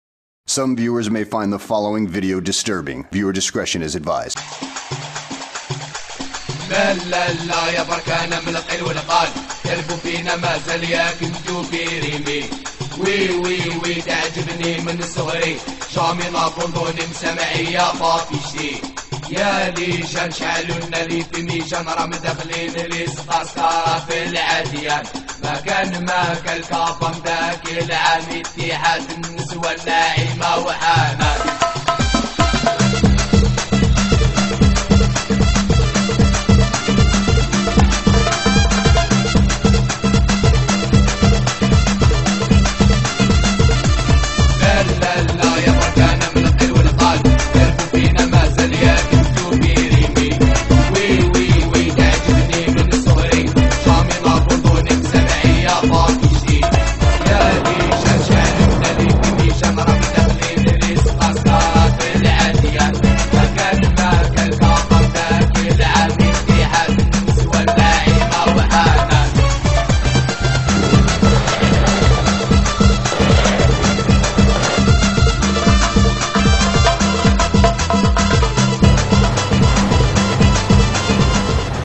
Syrian soldier sends a video message
Battles for Syria  - May 15th 2018 - Images from the anti-ISIS operation in South Damascus

SYRIA-RUSSIAN JETS,BATTLE-TANKS & SPECIAL FORCES OF THE S.A.A FINISH OFF ISIS IN AL-YARMOUK RUINS

Updates from the anti-ISIS operation in South Damascus - May 14th 2018 - Syria.